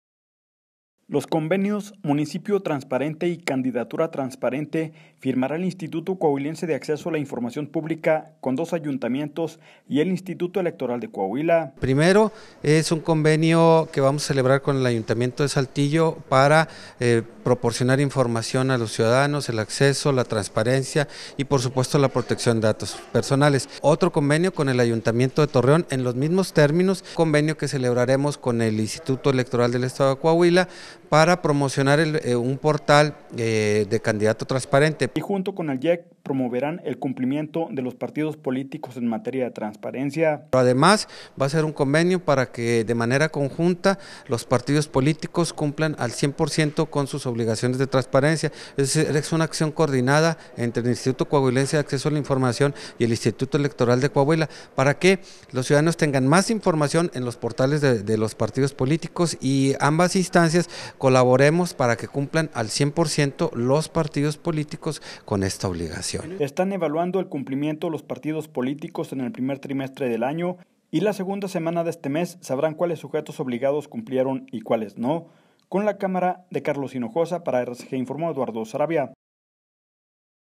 Firmara ICAI convenios con IEC y municipios
Los convenios Municipio Transparente y Candidatura Transparente firmará el Instituto Coahuilense de Acceso a la Información Pública con dos ayuntamientos y el Instituto Electoral de Coahuila.